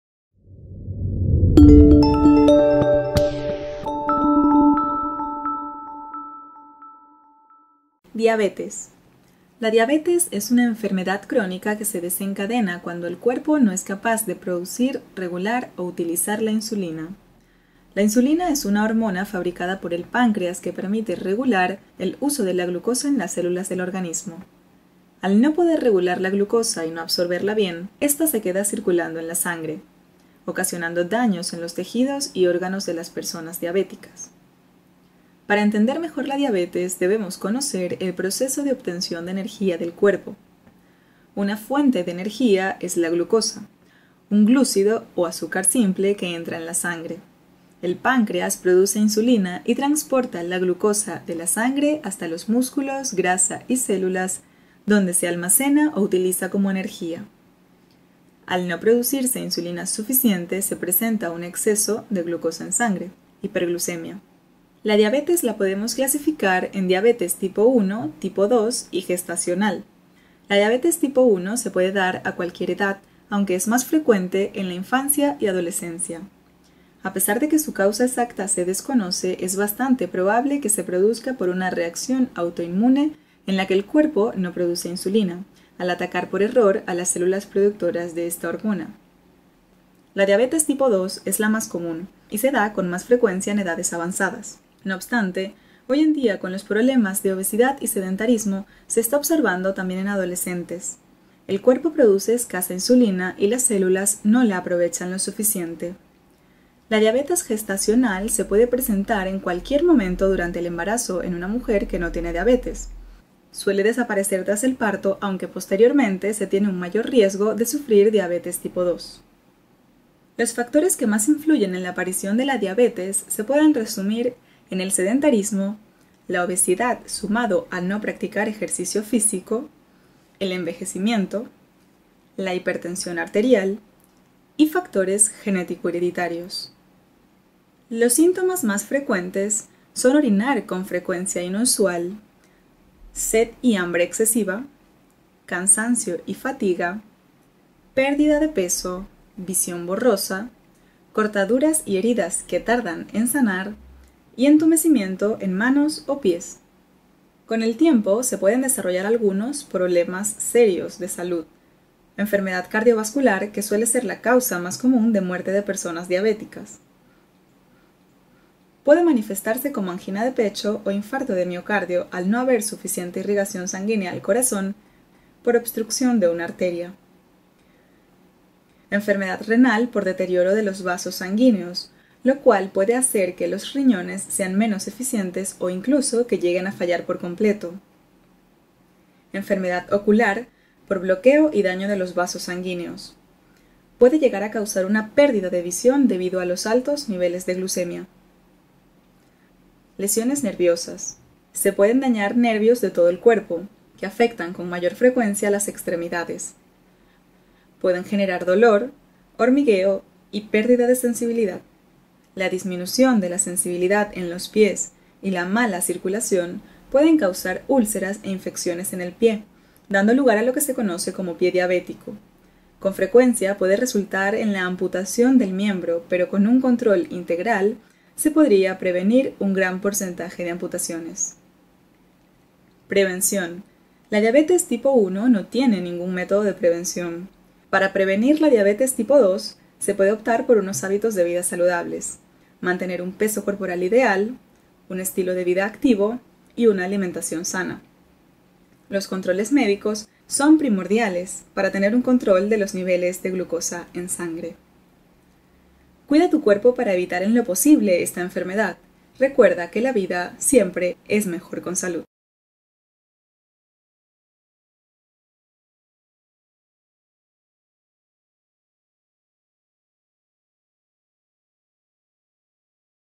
La diabetes - Tipos y recomendaciones para tu tratamiento
Hoy en Mejor con Salud te vamos a contar todo lo que necesitas saber sobre la diabetes, una enfermedad por la que se descompensan los niveles de azúcar en sangre y que condiciona la vida del paciente, ya que debe seguir un tratamiento de por vida. ¿Quieres saber cómo puedes regular la glucosa en sangre? ¡Descúbrelo en este artículo

Si quieres prevenir su aparición, te contamos cómo hacerlo en este artículo
Descubre cómo puede ayudarte el aloe vera a tratar esta dolencia

Recibirás tres vídeos cada semana.